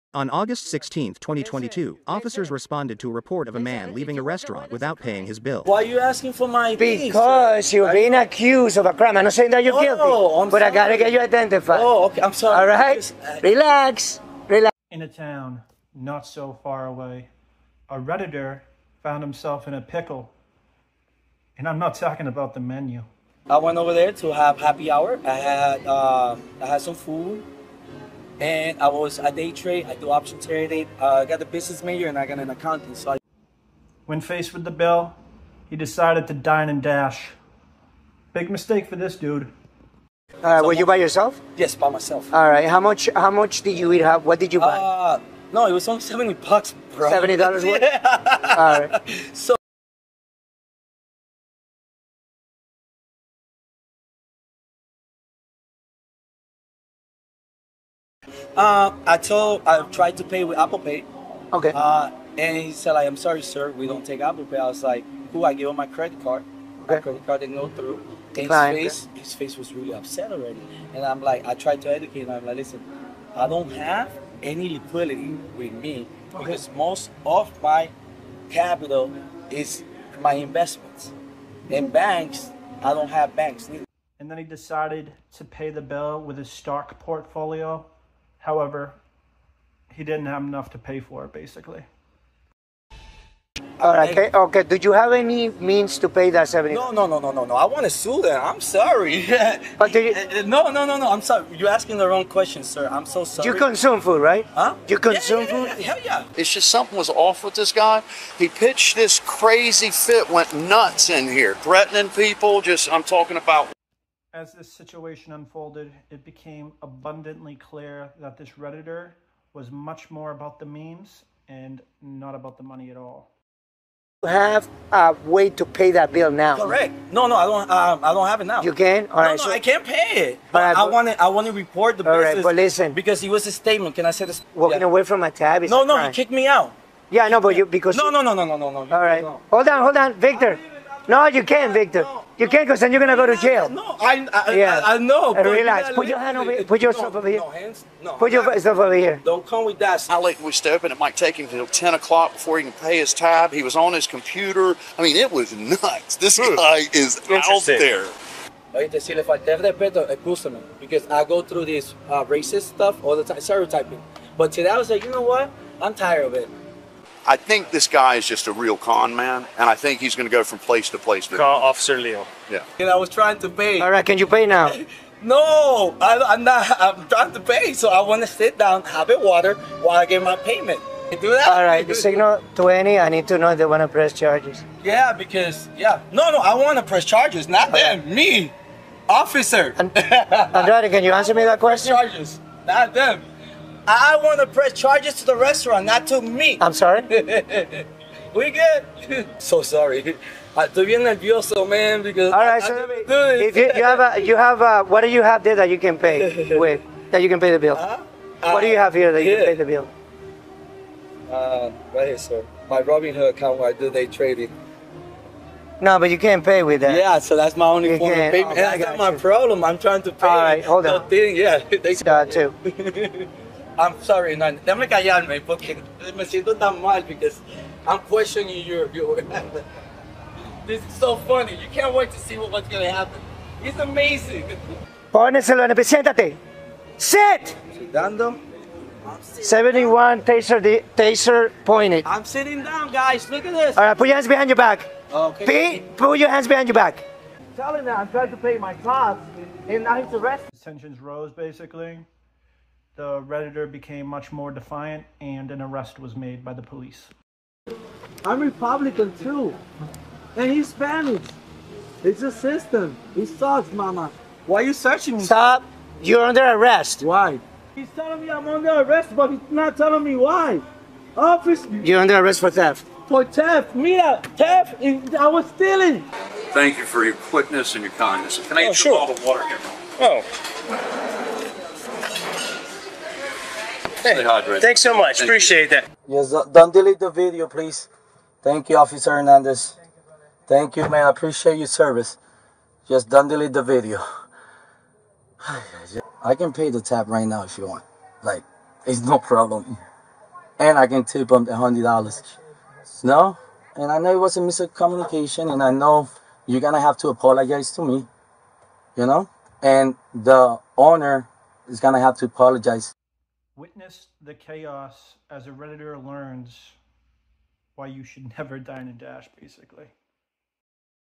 Crazy Redditor Dine & Dash Arrest Footage | Cringe Drama
Friend me on Instagram to spread the word of failed policies

So, what we have here is the average Redditor dining and dashing because they're unemployed. Let's be real, they dine and dash, pretend to invest their money, but end up losing it actively. They seem clueless about life because they lack intelligence.

Check out this wild guy losing it, getting arrested, handcuffed, and then crying. Why did this happen? Because he attempted to skip the bill, aka dine and dash, at a Florida restaurant. The police were called, and the guy was just incredibly annoying and cringe.